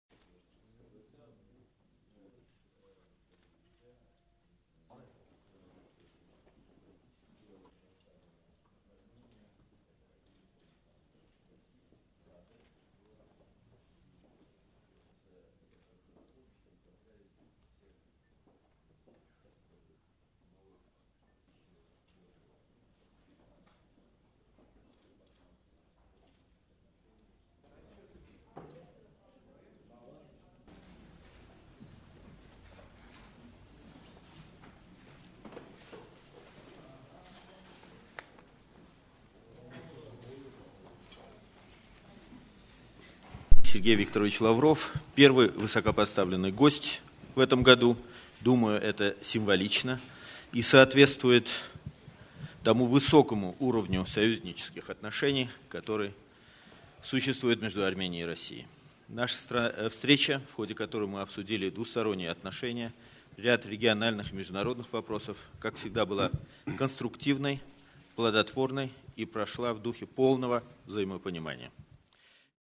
Երեւանում է Ռուսաստանի արտգործնախարարը
Ռուսաստանի արտգործնախար Սերգեյ Լավրովն այսօր մամուլի ասուլիսում ասաց, որ այցը Երեւան նախօրոք ծրագրված էր եւ կապված չէ Մոսկվա Թուրքիայի վարչապետ Էրդողանի այցի հետ: